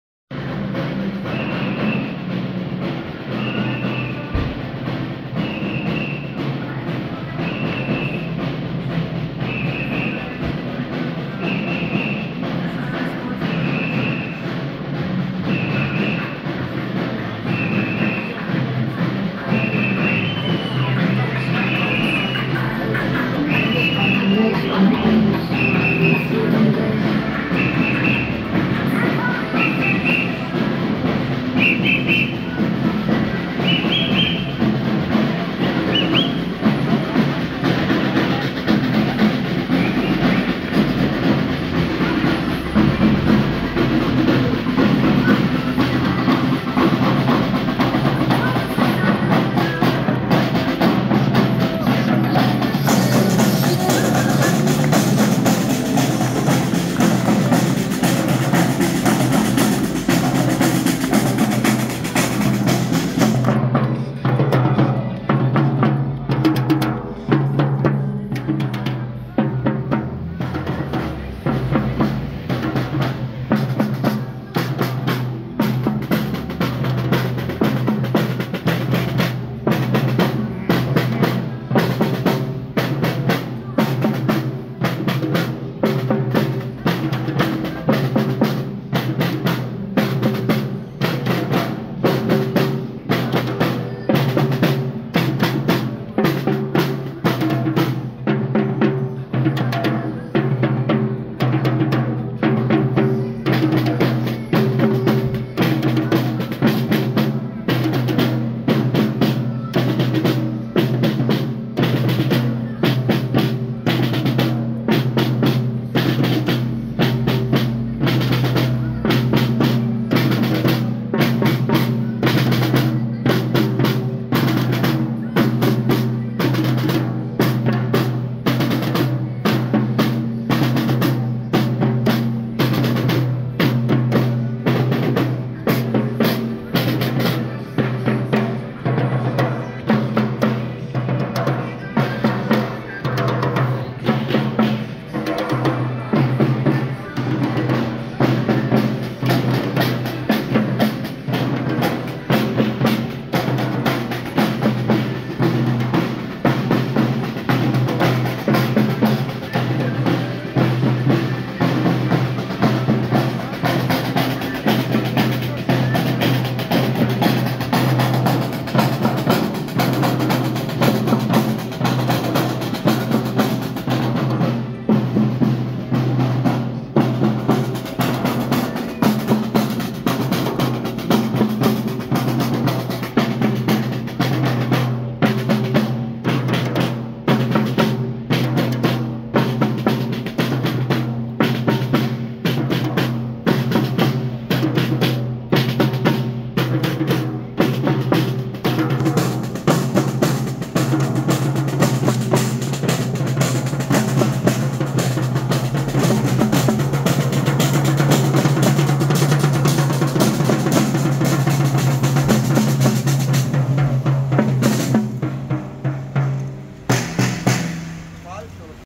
Demo für Frieden, Freiheit und ein friedliches selbstbestimmtes Leben am 09.10.2023 in Bamberg
Die 135 Spaziergänger bedanken sich bei den Freiheitstrommlern mit Applaus